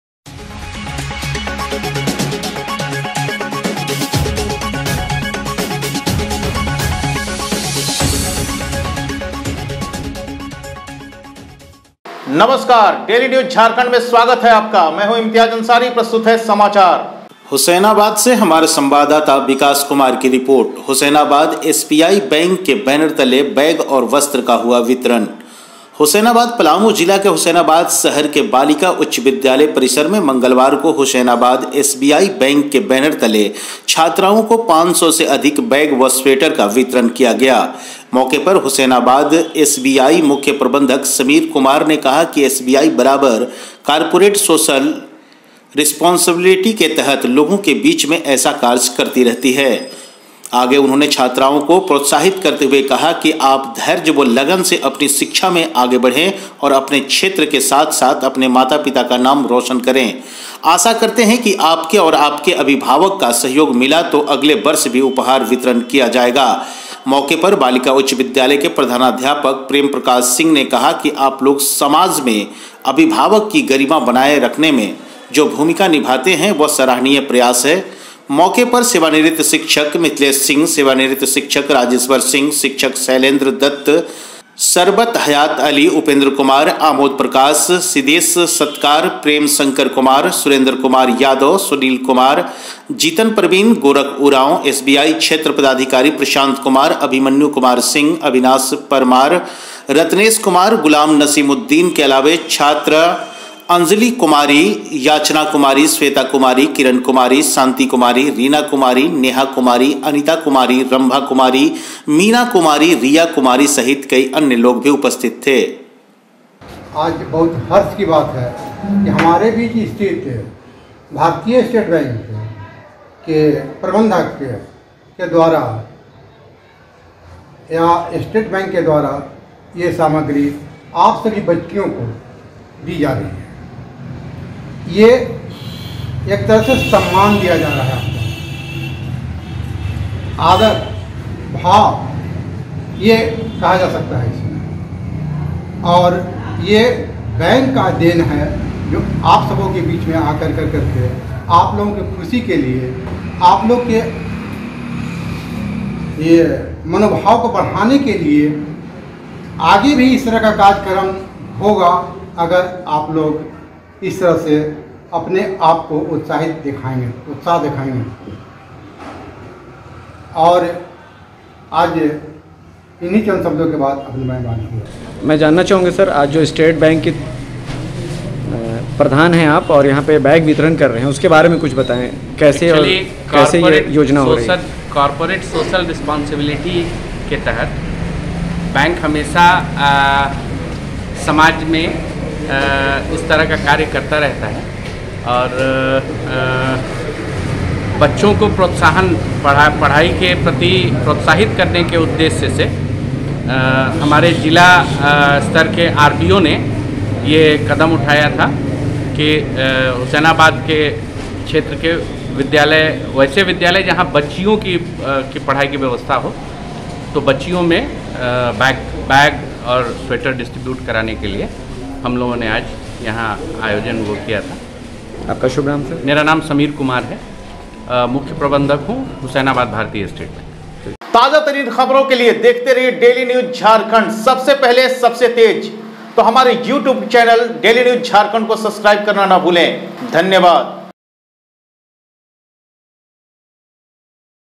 हुसैनाबाद | एसबीआई बैंक के बैनर तले बैग और वस्त्र का हुआ वितरण | Daily News Jharkhand
हुसैनाबाद से संवाददाता विकास कुमार की रिपोर्ट

हुसैनाबाद एसबीआई बैंक के बैनर तले बैग और वस्त्र का हुआ वितरण

हुसैनाबाद: पलामू जिला के हुसैनाबाद शहर के बालिका उच्च विद्यालय परिसर में मंगलवार को हुसैनाबाद एसबीआई बैंक के बैनर तले छात्राओं को 500 से अधिक बैग व स्वेटर का वितरण किया गया। मौके पर हुसैनाबाद एसबीआई मुख्य प्रबंधक समीर कुमार ने कहा की एसबीआई बराबर कॉरपोरेट सोशल रिस्पांसबलिटी के तहत लोगों के बीच में ऐसा कार्य करती रहती है । आगे उन्होंने छात्राओं को  प्रोत्साहित करते हुए कहा कि आप धैर्य व लगन से अपनी शिक्षा में आगे बढ़े और अपने क्षेत्र के साथ अपने माता - पिता का नाम रौशन करें। आशा करते कि आपके और आपके अभिवावक का सहयोग मिला तो अगले वर्ष भी उपहार वितरण किया जाएगा। मौके पर बालिका उच्च विद्यालय के  प्रधानाध्यापक प्रेम प्रकाश सिंह ने कहा कि आप लोग समाज में अभिभाव की गरिमा बनाए रखने में जो भूमिका निभाते है वह सराहनीय प्रयास है। वहीं  मौके पर सेवानिवृत्त शिक्षक मिथिलेश सिंह, सेवानिवृत्त शिक्षक राजेश्वर सिंह, शिक्षक कौशलेंद्र दत्त, शरबत  हयात अली, उपेन्द्र कुमार,अमोद प्रकाश, सिदेश सत्कार प्रेम शंकर कुमार, सुरेंद्र कुमार यादव, सुनील कुमार, जीनत प्रवीण, गोरख उरांव,  एसबीआई क्षेत्र  पदाधिकारी प्रशांत कुमार , अभिमन्यु कुमार सिंह अविनाश परमार,  रत्नेश कुमार, गुलाम नसीमुद्दीन  के अलावे छात्रा अंजली कुमारी , याचना कुमारी, स्वेता कुमारी, किरण कुमारी,शांति कुमारी,रीना कुमारी, नेहा कुमारी, अनिता कुमारी , रंभा कुमारी, मीणा कुमारी, रिया कुमारी सहित अन्य लोग मौजूद थे।